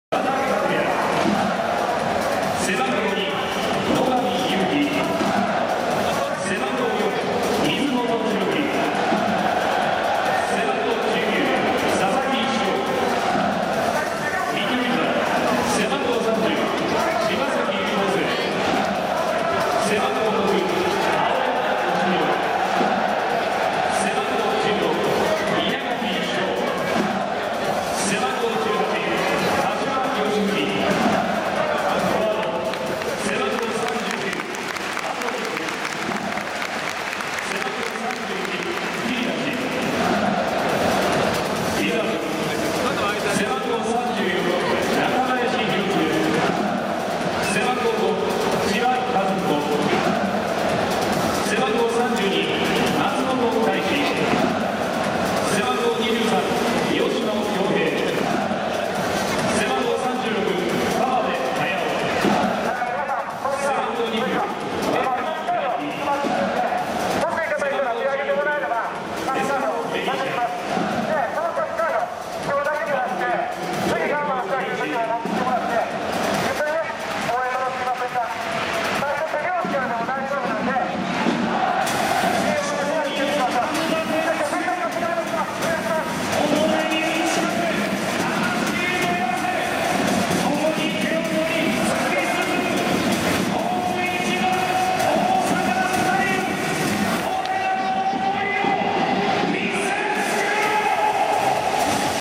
ガンバサポからサンフレに送られた最大のブーイングは青山敏弘
2018年09月29日（土）
第28節   ガンバ大阪vsサンフレッチェ広島

0 前 半 0
1 後 半 0
1 合計  0

得点：71 ファン・ウィジョ
警　告：90+3 柏好文

日時:09月29日(土)16:05 キックオフ
天気・気温:曇一時雨、弱風、気温20.5℃、湿度60％
場所:パナソニックスタジアム吹田
入場者数:34,660人
芝状況:全面良芝、水含み
主審:飯田淳平
副審:越智新次、大塚晴弘
第4の審判:大坪　博和